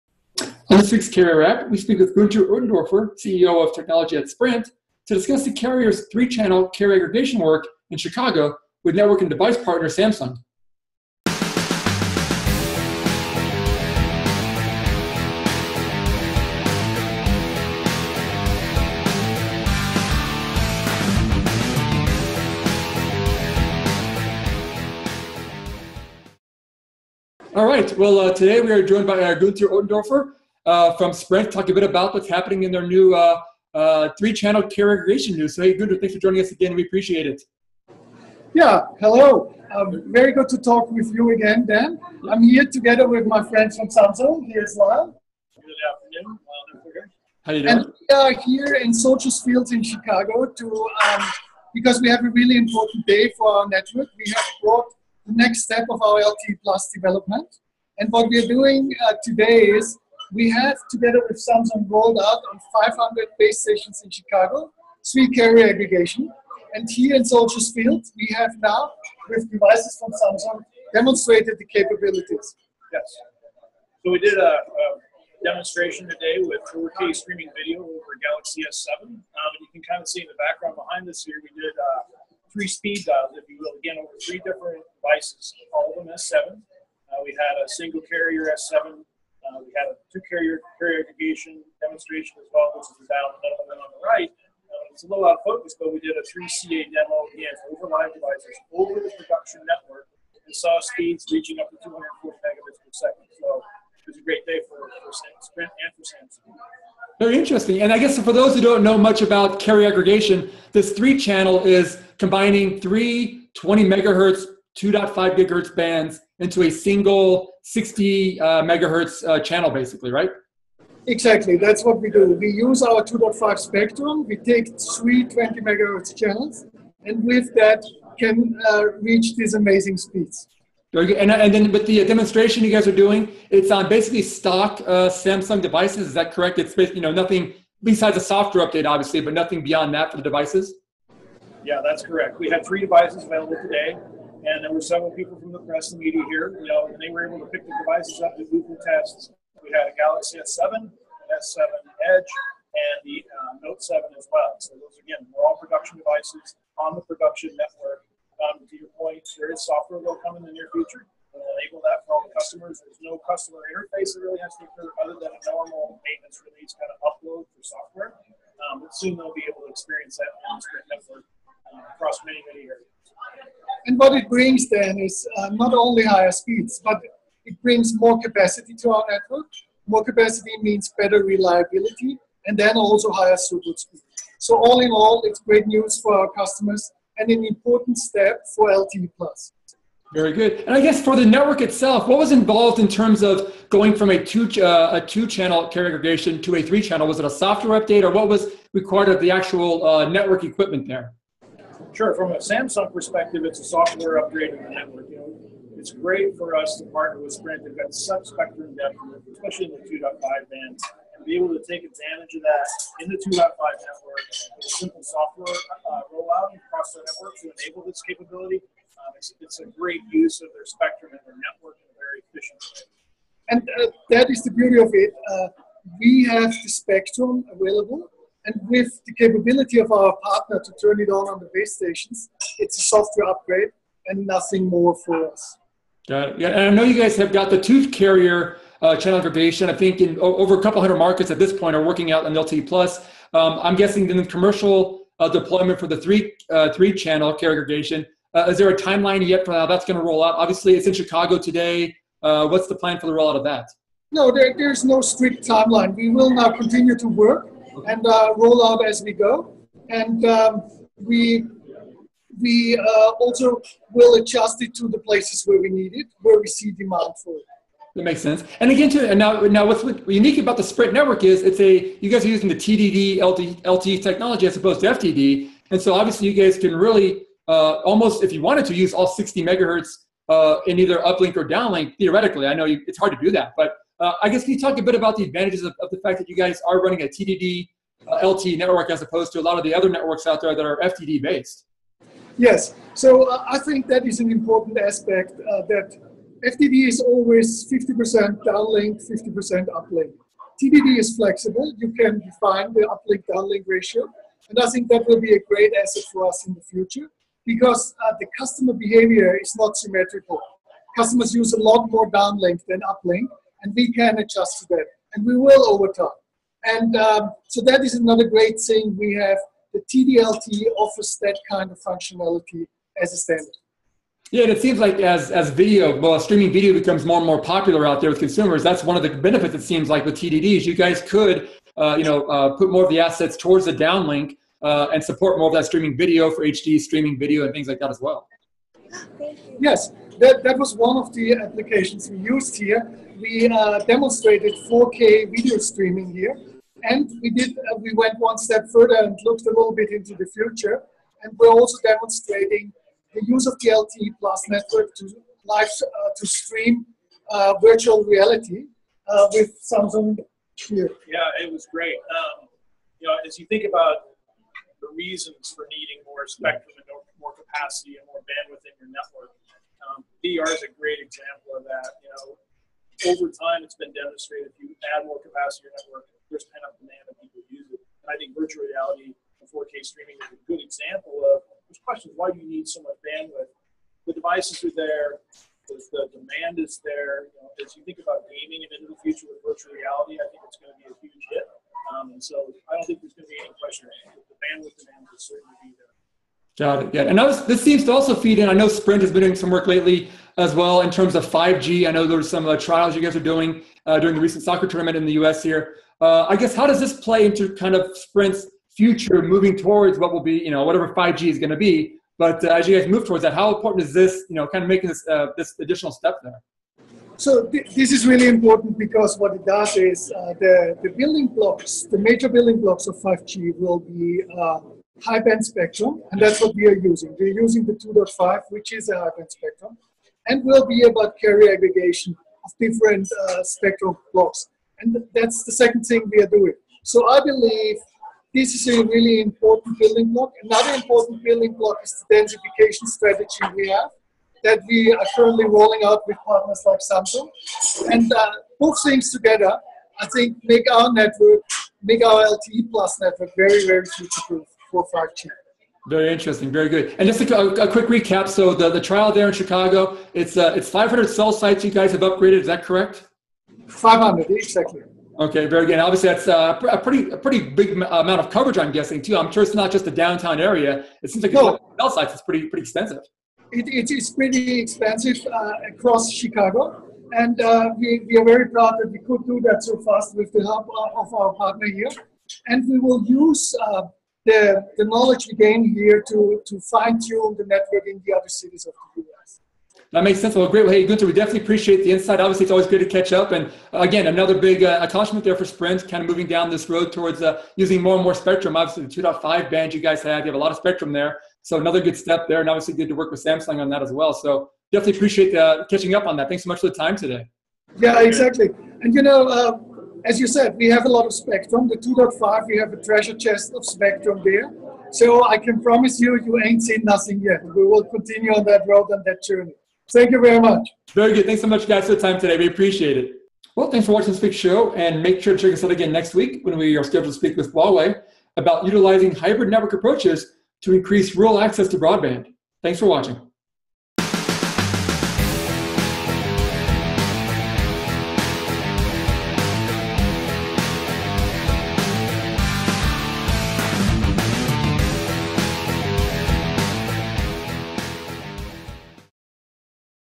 Sprint pushes 3CCA for LTE networks in Chicago, Kansas City – Carrier Wrap Ep. 43
Sprint recently made an aggressive move to take advantage of its deeps 2.5 GHz spectrum holdings by launching network trials of three-channel carrier aggregation technology in Chicago and Kansas City.

The carrier said it had deployed 3CCA capabilities across 500 cell sites in Chicago, with each site supporting three 20-megahertz channels of 2.5 GHz spectrum. The 60-megahertz of total spectrum is said to provide download speeds of more than 230 megabits per second using commercial Samsung devices like the Galaxy Note 7, Galaxy S7 and Galaxy S7 Edge.

That was followed by a similar move in Kansas City, Kansas, with a promise to deploy upgraded technology based on the LTE-Advanced standard to some 100 cell sites in the city. The carrier said the move, in conjunction with Nokia, produced similar peak data rates during a demo inside Kauffman Stadium prior to the big-league baseball game between the hometown Royals and New York Yankees.

Prior to the network trials, Sprint has over the past several months been discussing 3CCA lab trials using various Samsung, HTC and LG devices showing network speeds of up to 300 Mbps.

The network trials came on the heels of Verizon Wireless moving on a broad roll out of both two-channel and three-channel carrier aggregation technology using its 700 MHz, 1.7/2.1 GHz and 1.9 GHz spectrum holdings. The LTE Advanced service is said to support network speeds 50% faster than its basic LTE network – so figure download speeds of between 7.5 Mbps and 18 Mbps – across more than 450 markets.

On this week’s Carrier Wrap we speak with Guenther Ottendorfer, COO of technology at Sprint, to discuss the carrier’s 3CCA work in Chicago with Samsung, and what the carrier has in mind for commercial deployments.

Thanks for watching this week’s show and make sure to check us out again next week when we speak with Huawei about utilizing hybrid network approaches to increase rural access to broadband